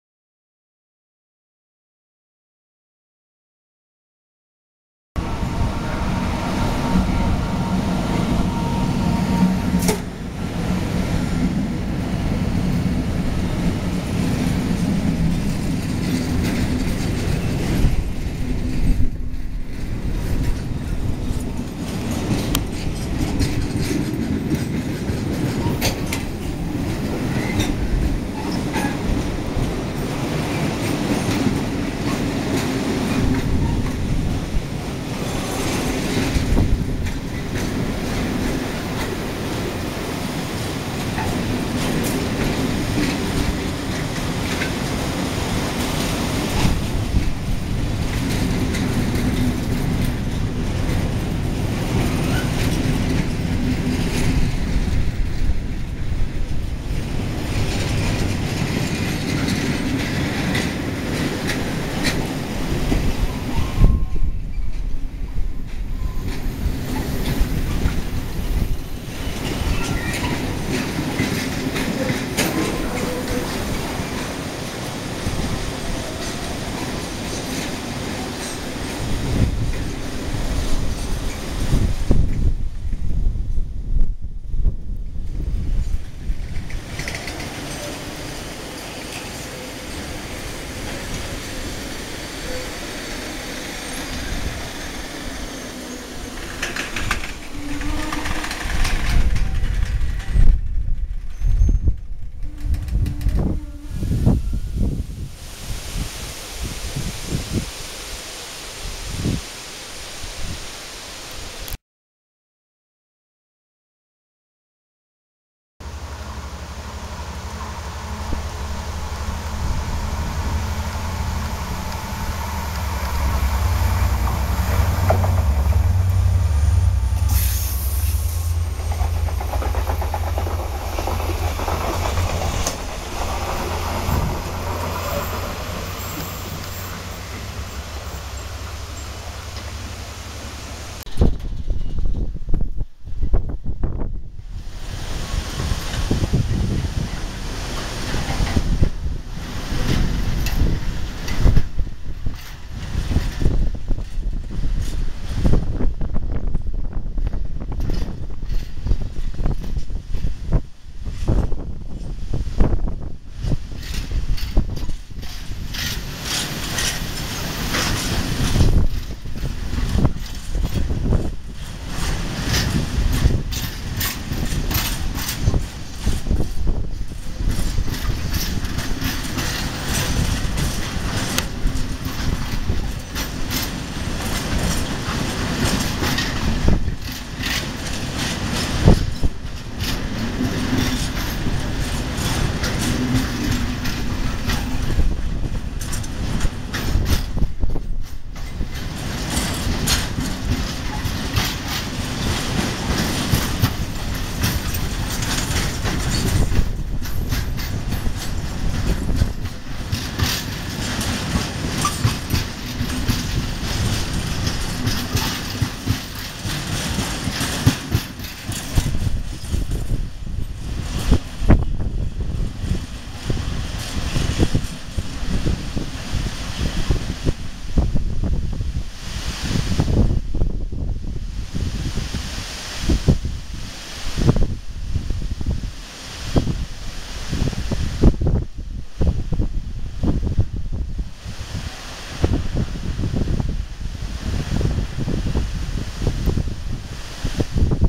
269.853-8TANDEM+TECO. Parada en Aranjuez.
01/11/2012; Una vez más, para haber sido día de todos los santos, para mi fue el día de los "santos trenes". La verdad que la mañana para mí fue menos movida que por la tarde. Hay que decir que con el viento que hacía y el frio no había ganas de salir de casa, pero entonces me hubiera perdido, entre otros mercancías que ví ese día, la parada de este TECO.